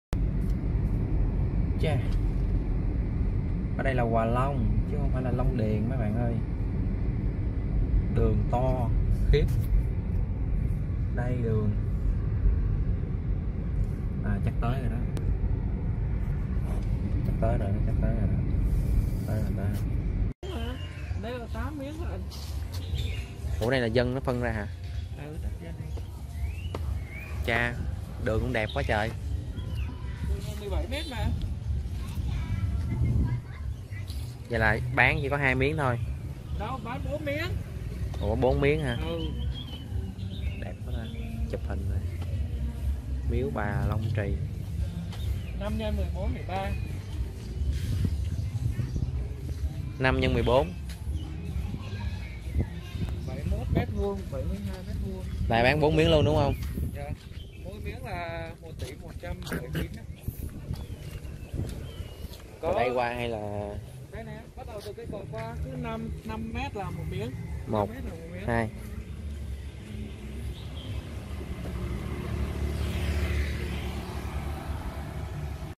Phongcomputer | Đất nền 5x14.5 Thổ Cư 100%
Còn 4 lô đất nền long điền khu dân cư Giá Đầu Tư ( Hỗ Trợ Vay 25 Năm 70% Giá Trị )
Trục đường Chính
🏠Diện tích : 100m2 thổ cư
📕 Đẫ có sổ - Thoát hiểm sau lưng 5m-10m
🔑 Sang tay trong 1 nốt nhạc
🚘 ngay trường học , công viên , chơ, khu mua bàn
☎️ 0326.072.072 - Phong
💰Giá :1tyx tr/Nền/100m2 (xx Tùy vào mối quan hệ )
Hỗ Trợ Vay Ngân Hàng 70% Giá Trị Nền